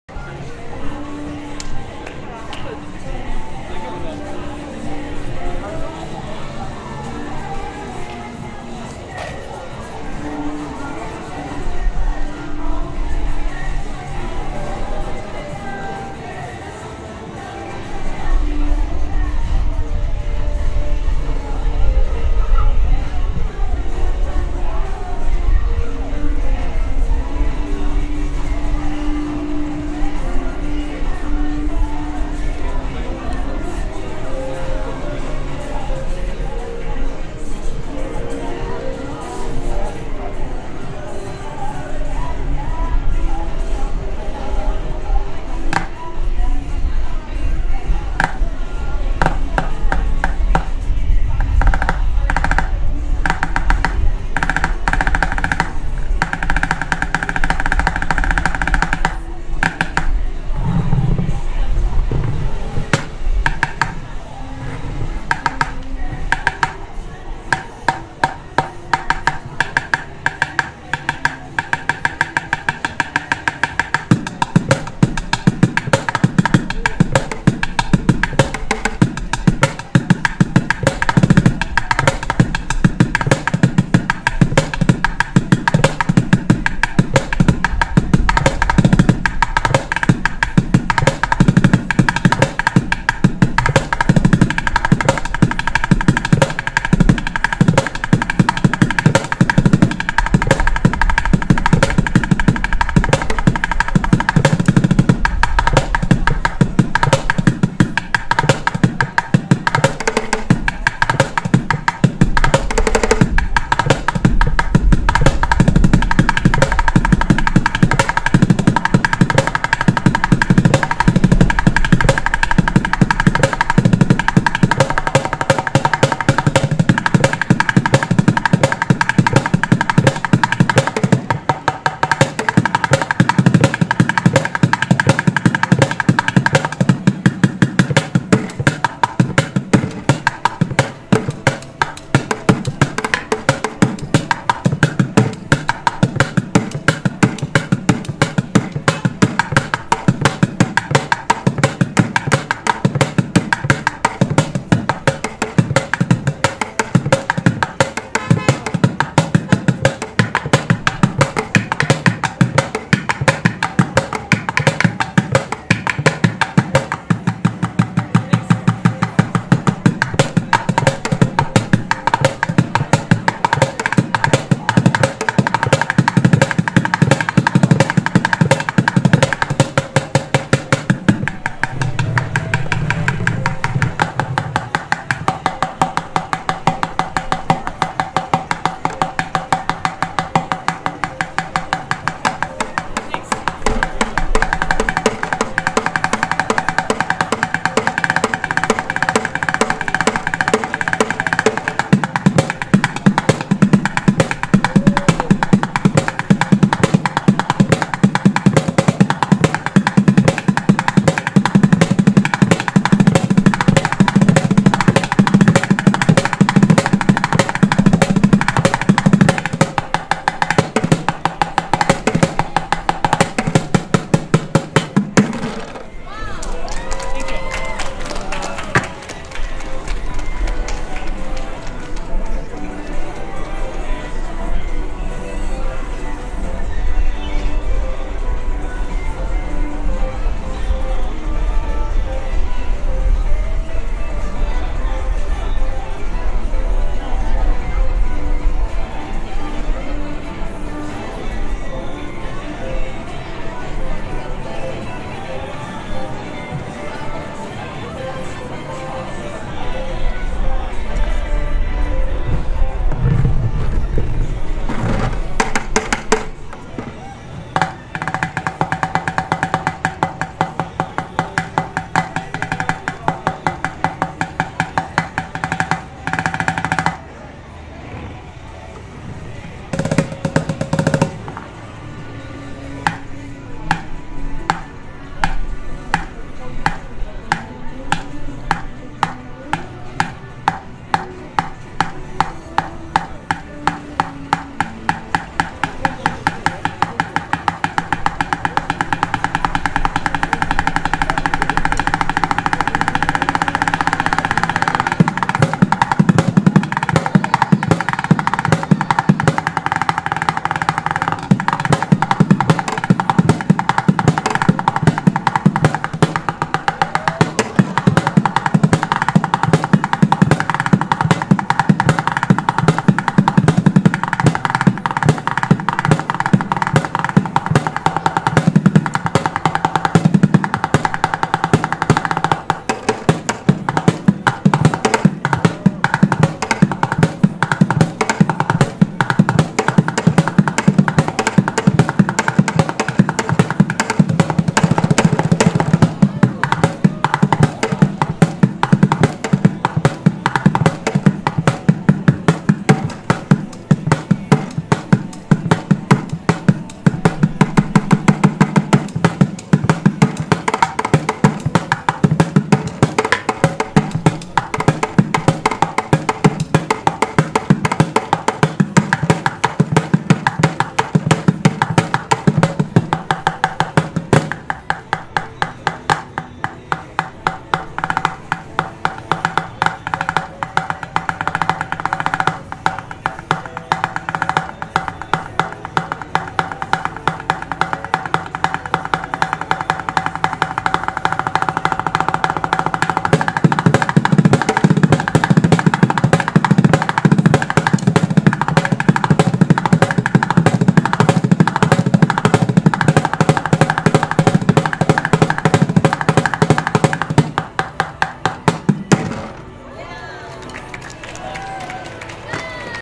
Starting Busking with no crowd
Its always fun to see a crowd form right in front of you.